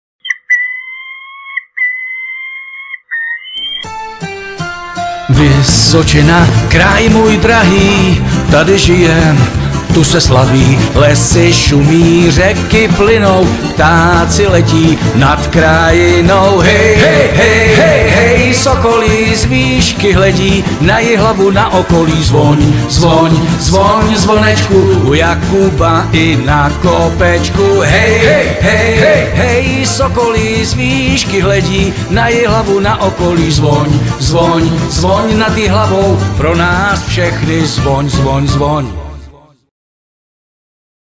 Sokol v Jihlavě
Sokol prý má dnes výročí, tak jsem si dovolil narychlo uplácat hudební dárek :-) ( zdroj videí youtube )